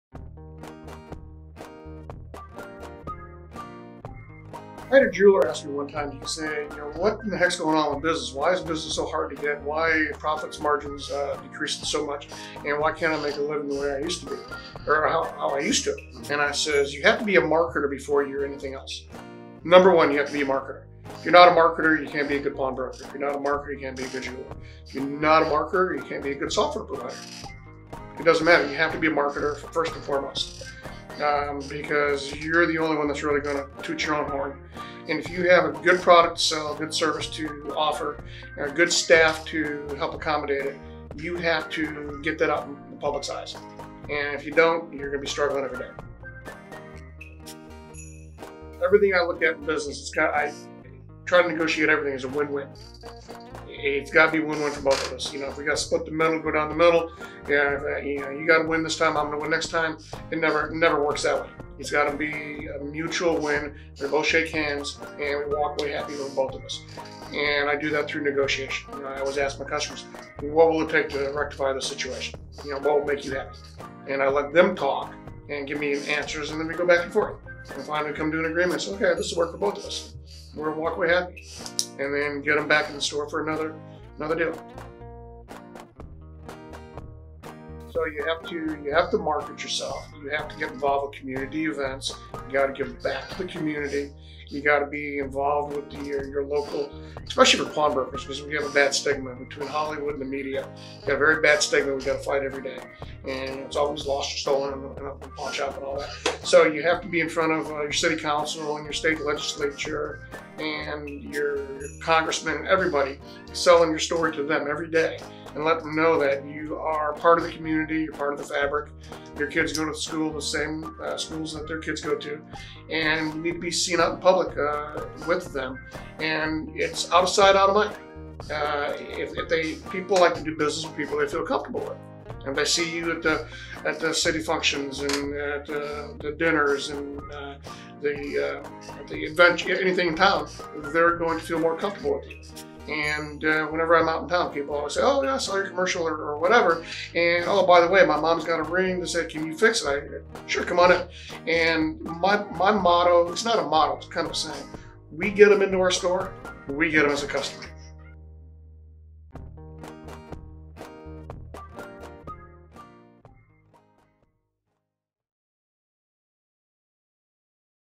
Broker-to-Broker: Perry Lewin of Decatur Jewelry and Pawn
Perry Lewin knows that in order to be a good pawn shop owner, you have to be part pawnbroker, part marketer, and part community representative.